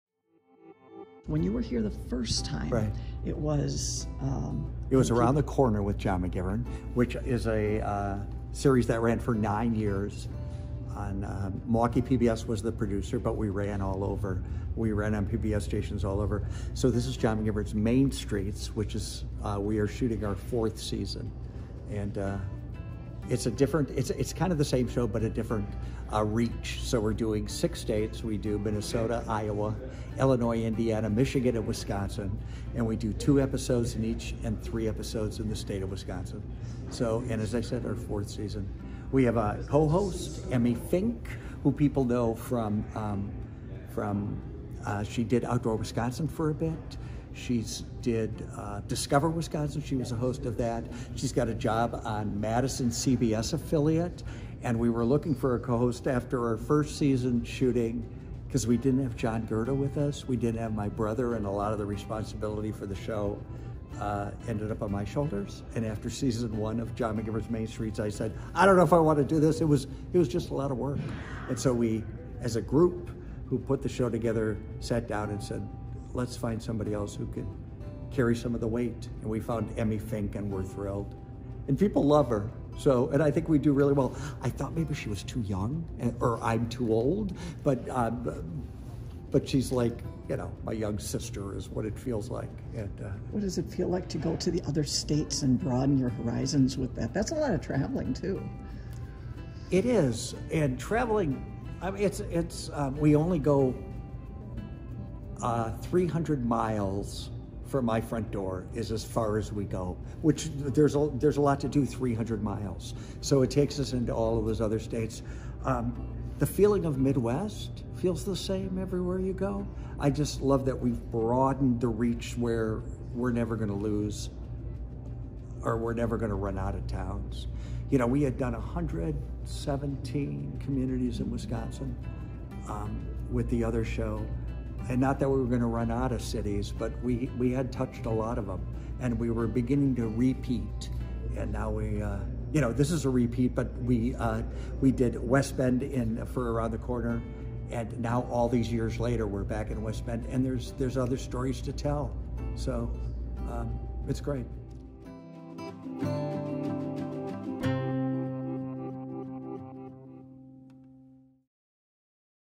One-on-one with John McGivern
It was the summer of 2015 when John McGivern was last in West Bend, Wi shooting Around the Corner with John McGivern. Now, nearly 10 years later McGivern is back with a new show, fresh cast, and spirited interviews that offer a unique take on a community.